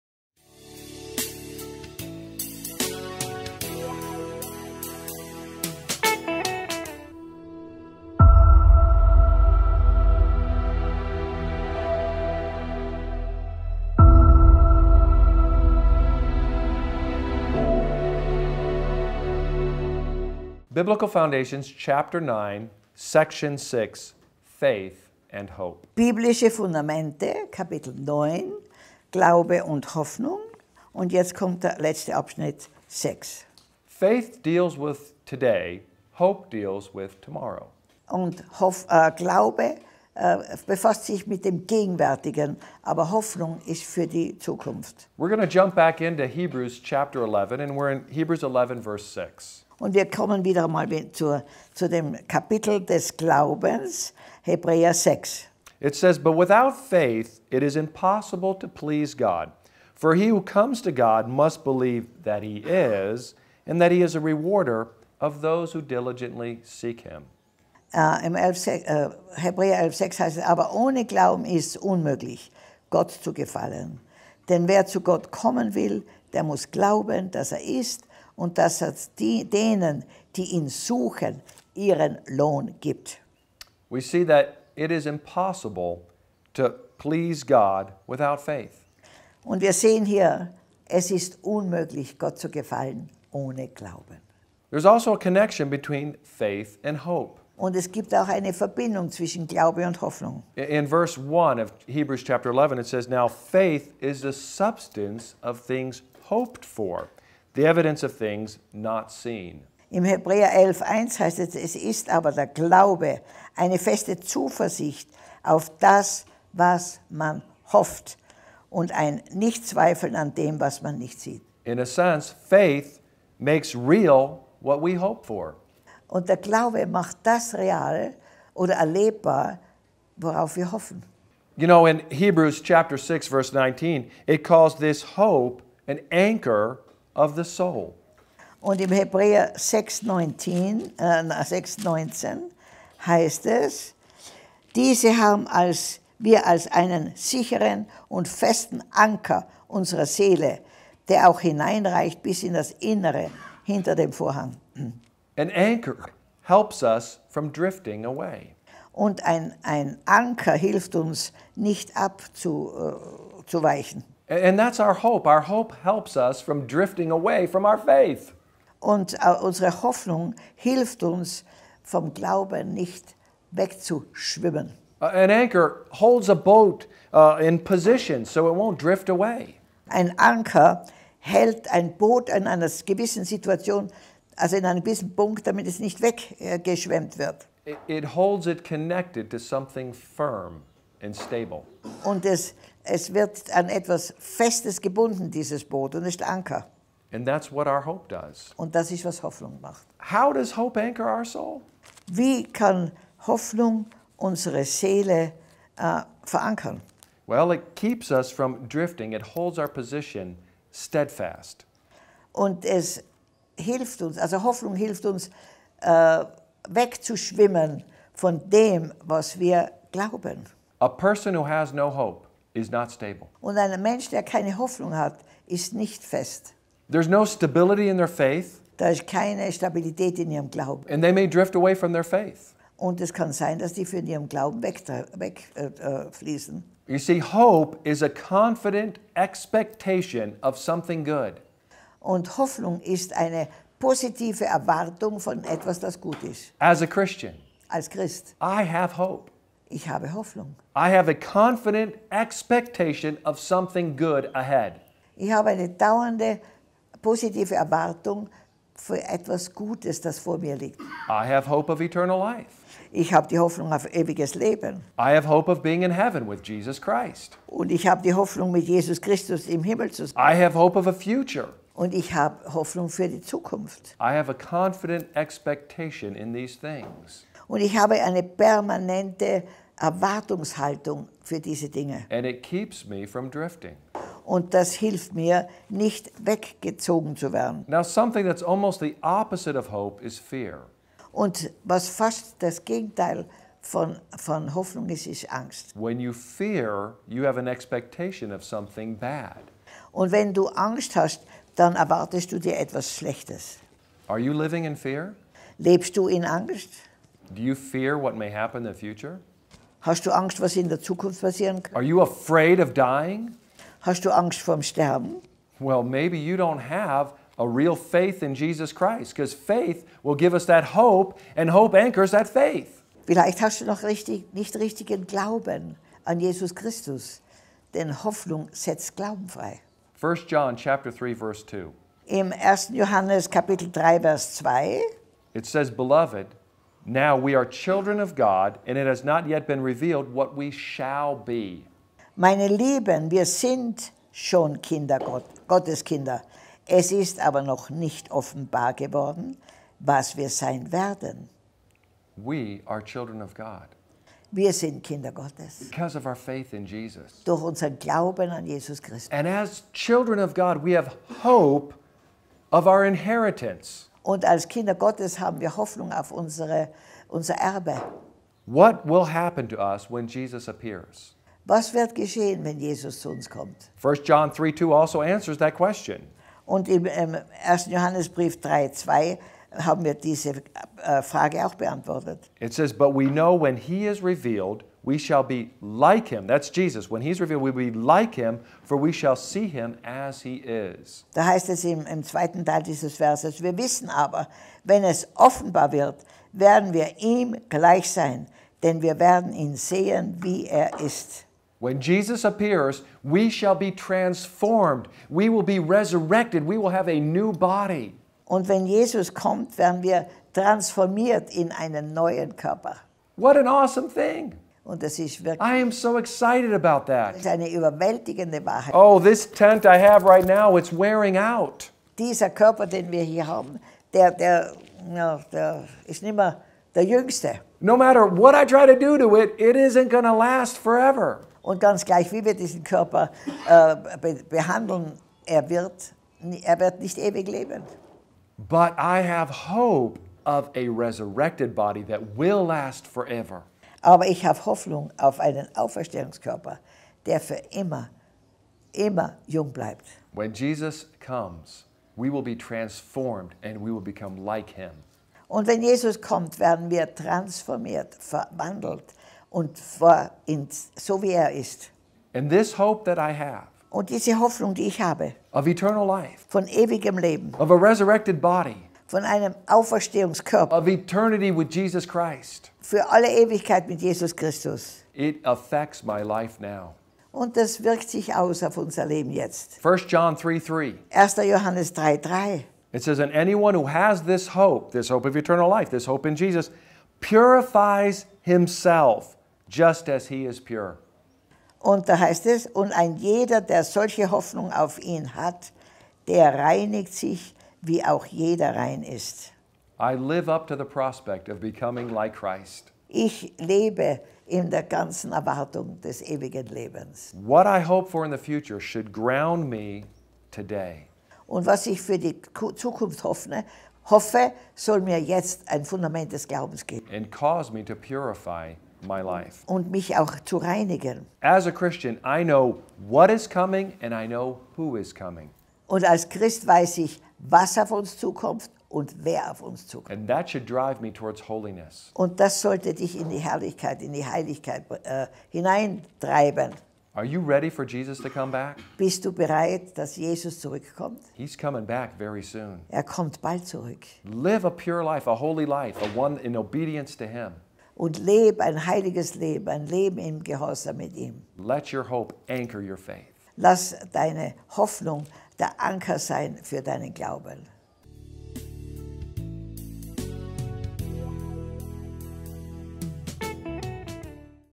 Pr. Isaac Wootton & Maria Prean - Biblical Foundation Discipleship Ch9/S6. Jüngerschaftskurs K9/T6
CHAPTER 9: Faith & Hope  / SECTION 6: Faith & Hope

Dig with Pastor Isaac Wootton (Messianic Jew from Israel) and Maria Prean into the biblical foundations of true discipleship. This is a course according to the same-named course book that stretches over 12 chapters with each 4 sections. Get the book as a PDF to download and print out

Every Sunday the next part will be uploaded, so that every month we are going through one chapter.

Join this online-course and be blessed as you become a true Disciple of Christ that disciples others!
KAPITEL 9: Glaube & Hoffnung/ TEIL 6: Glaube & Hoffnung

Tauche mit Pastor Isaac Wootton (Messianischer Jude aus Israel) und Maria Prean ein, in die biblischen Grundlagen von wahrer Jüngerschaft. Dies ist ein Kurs nach dem gleichnamigen Kursbuch, das aus 12 Kapiteln mit jeweils 4 Abschnitten (Teilen) besteht. Das Buch kannst du unter folgendem Link als PDF (auf Deutsch) herunterladen und ausdrucken:

Jeden Sonntag laden wir einen weiteren Teil hoch, sodass wir in einem Monat ein Kapitel durchmachen.

Nimm an diesem Online-Kurs teil und sei gesegnet, indem du ein wahrer Jünger Christi wirst, der andere ebenfalls zu Jüngern macht!